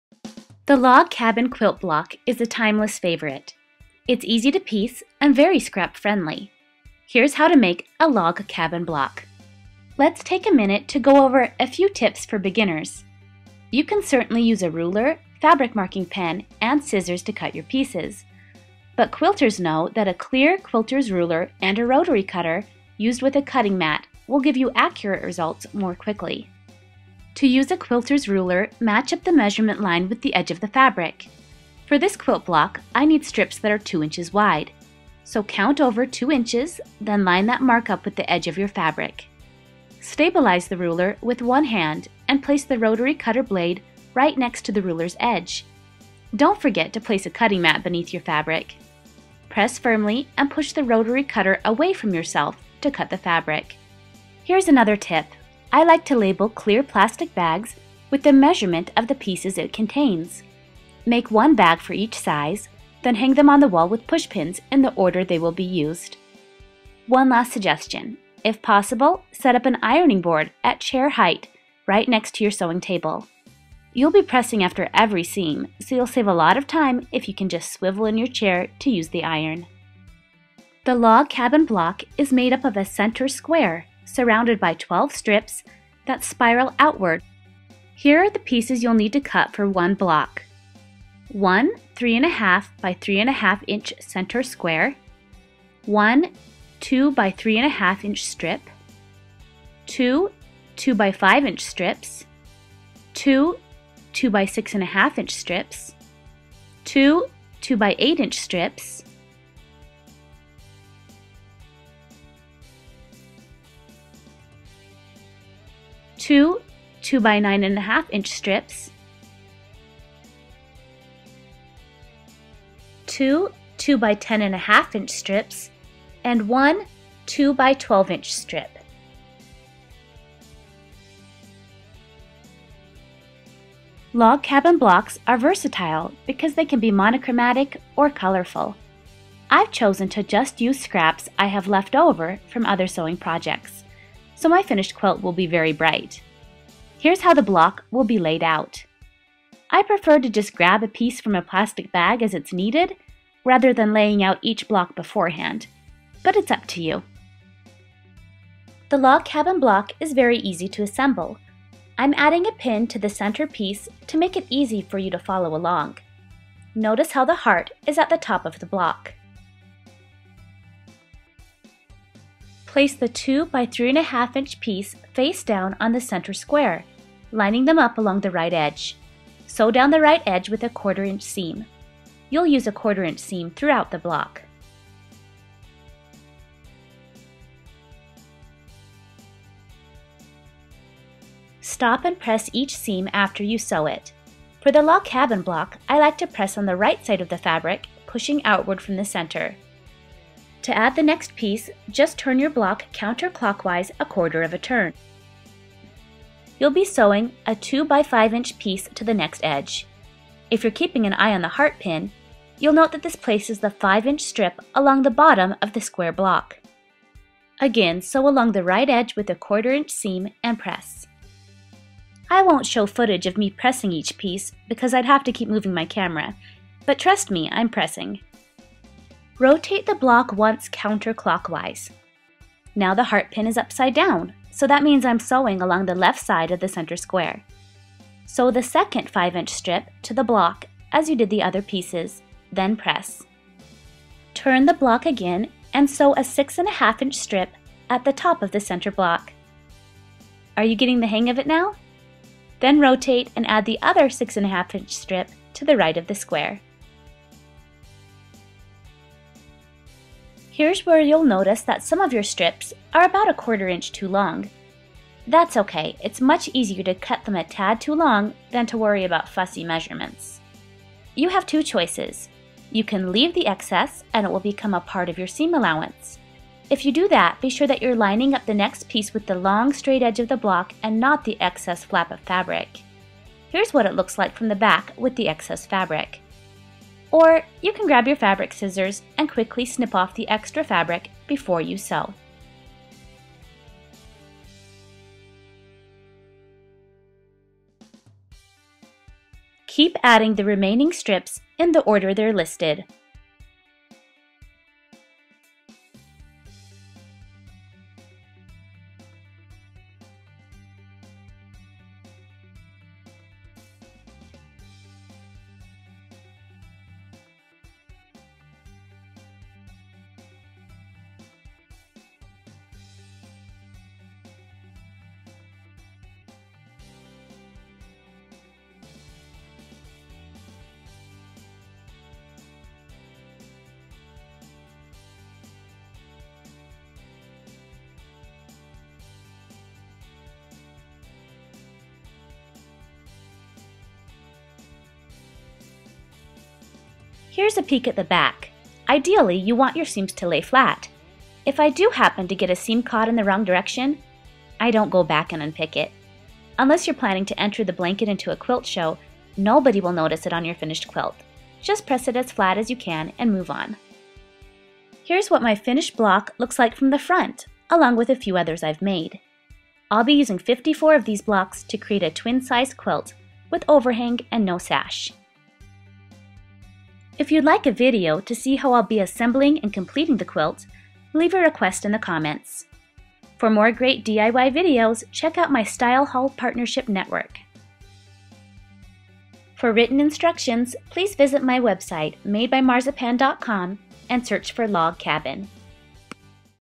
Log Cabin Quilt Block Tutorial
Learn to make a Log Cabin quilt block! The Log Cabin quilt block is a timeless favorite. It's easy to piece and very scrap-friendly. It's a good choice for beginning sewers. Watch this step-by-step tutorial to see how to make this simple, pretty quilt block.

Music used under license from Jewelbeat.com